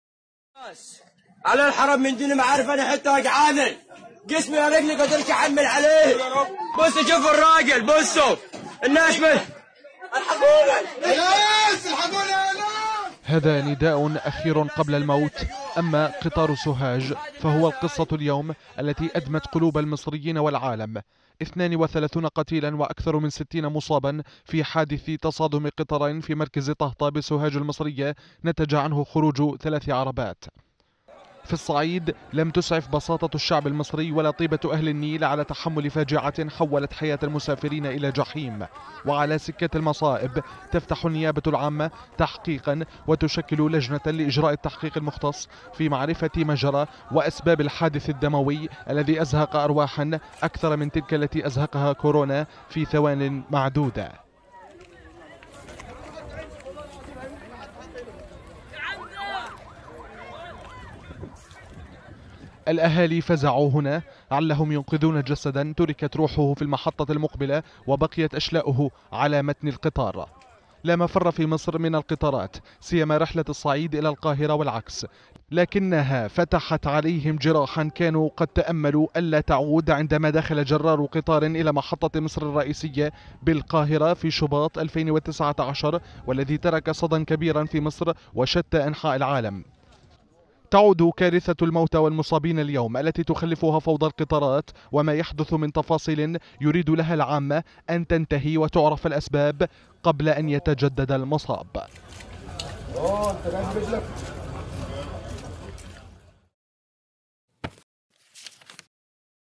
حادث سوهاج .. مشاهد تفجر غضب مصر والعالم | تقرير
عشرات القتلى والجرحى، راحوا ضحية حادث قطار_سوهاج، الذي أعاد للمصريين والعالم ما حدث في عام ألفين وتسعة عشر من كارثة دخول جرار قطار في محطة مصر الرئيسية، الأمر الذي وحد صوت المصريين بالمطالبة في إنهاء شلال دم حوادث القطارات.